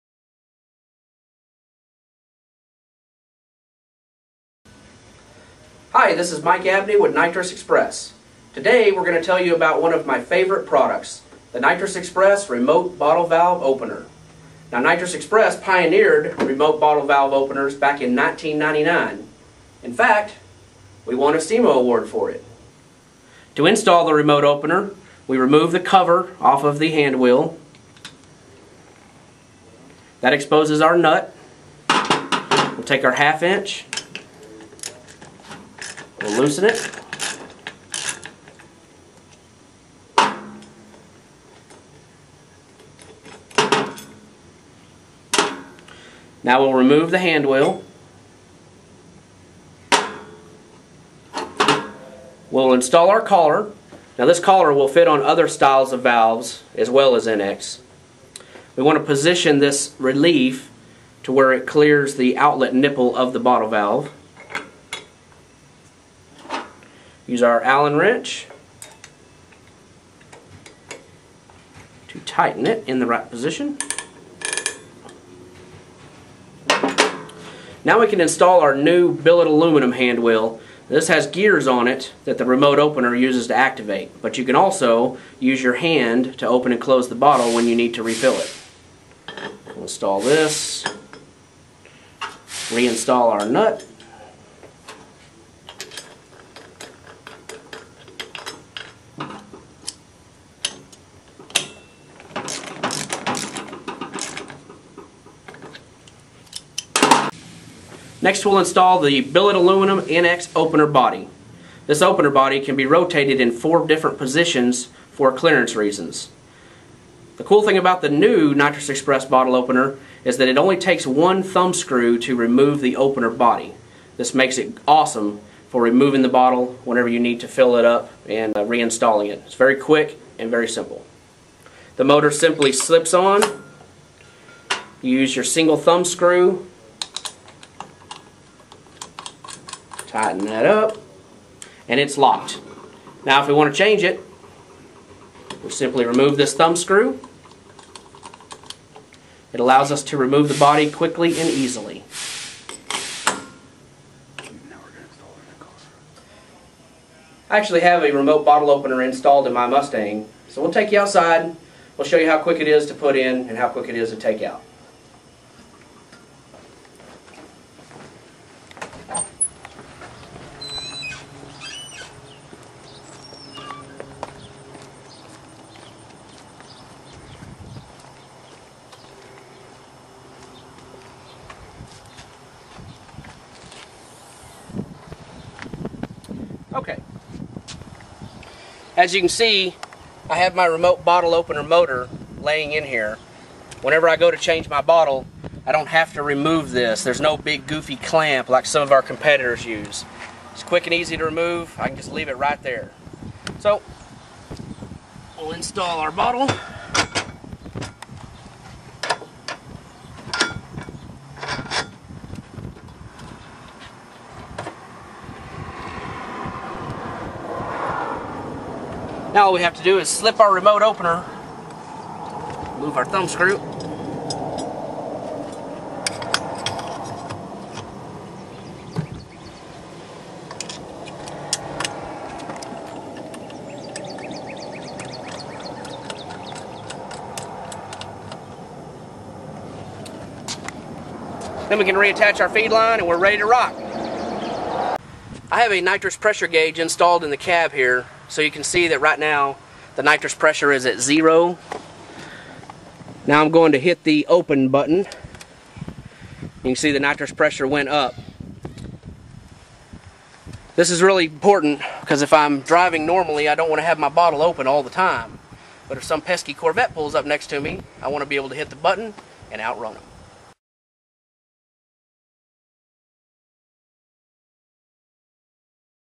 Nitrous Express Remote Bottle Valve Opener Demonstration
Here Mike Abney demonstrates our SEMA award winning Remote Bottle Valve Opener. This opener physically opens and closes the nitrous bottle valve with the push of a button.